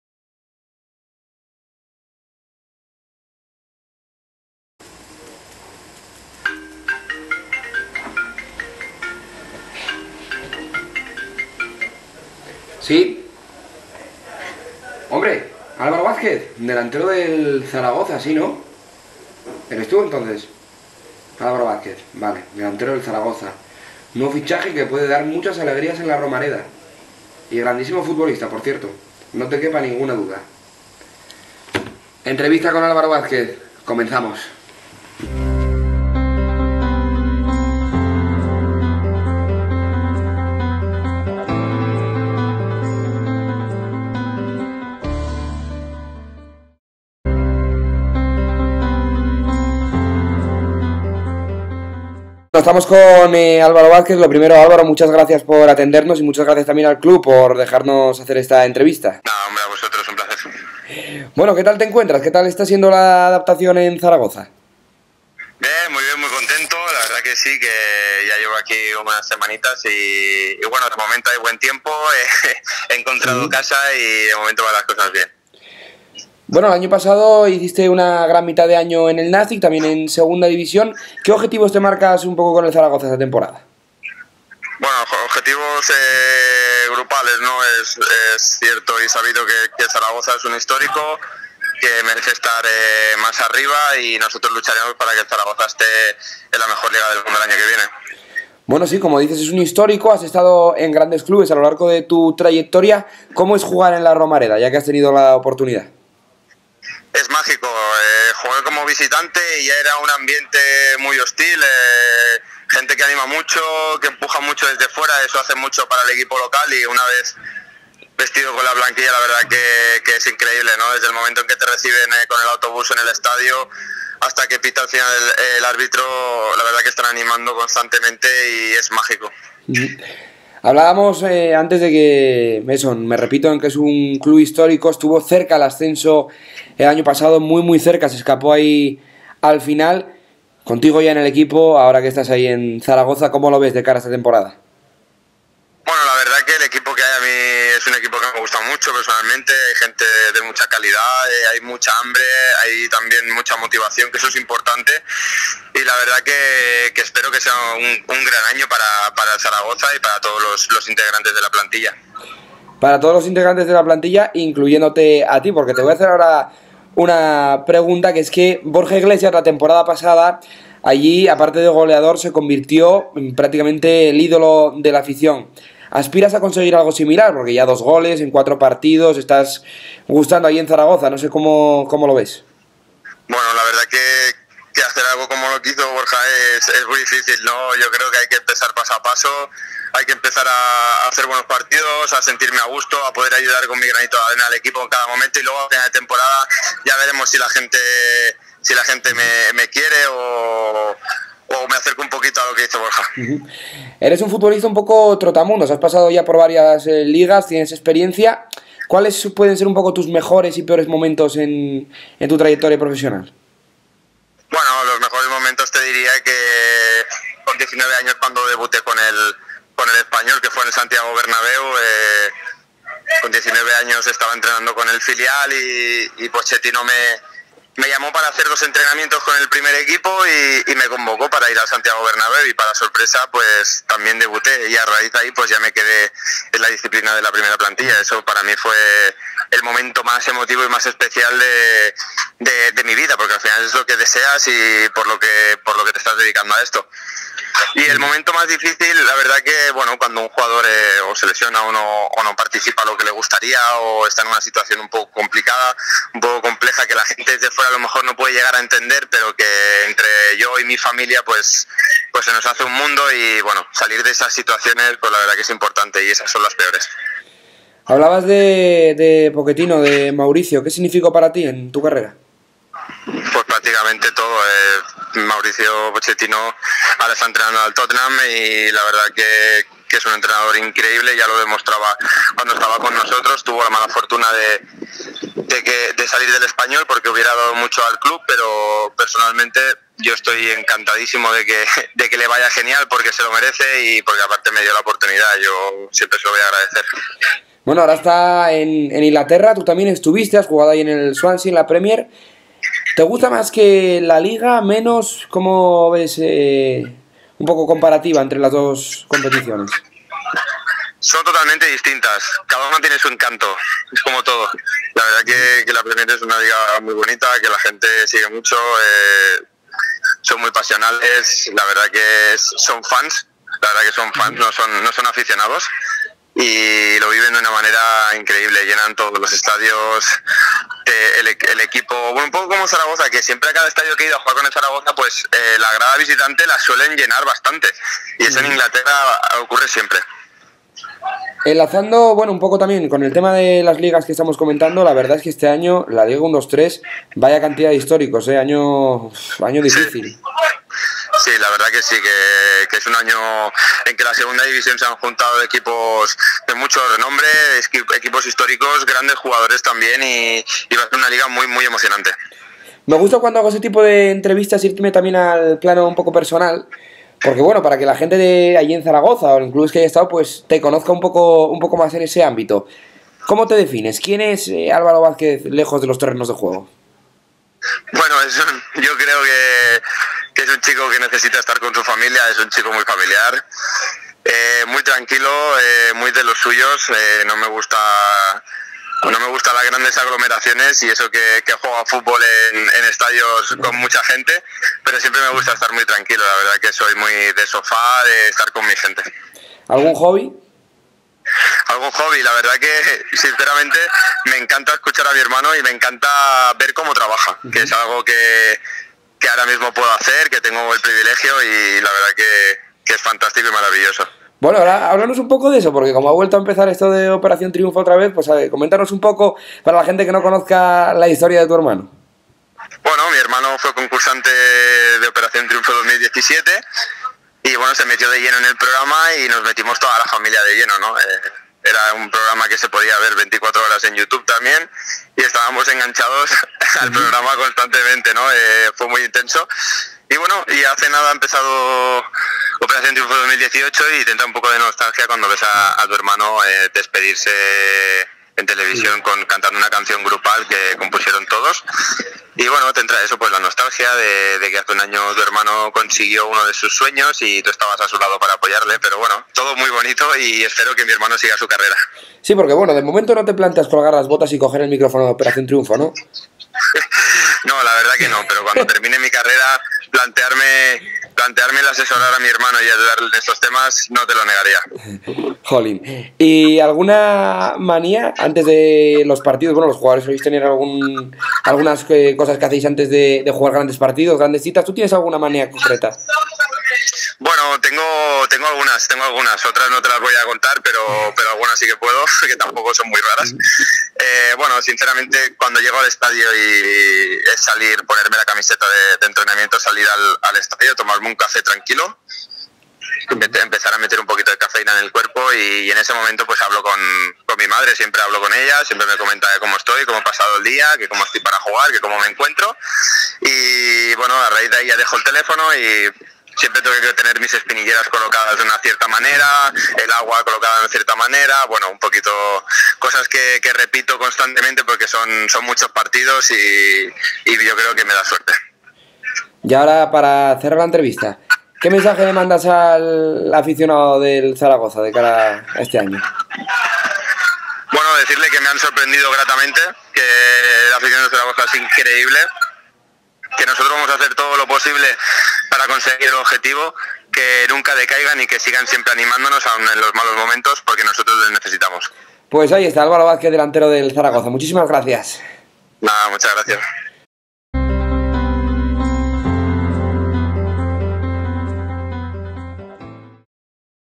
ÁLVARO VÁZQUEZ: "JUGAR EN LA ROMAREDA ES MÁGICO" | Entrevista
El nuevo delantero del RealZaragoza, Álvaro Vázquez, nos ha atendido para comentarnos cómo está siendo su adaptación, los objetivos de cara a esta temporada, la presión de sustituir a Borja Iglesias, contarnos alguna cosa de su vida personal... Todo esto y mucho más en el vídeo sobre LaLiga123.

 → Colaboran: @ViveSegunda, @lacaraBdeporte, @DeporSempre1.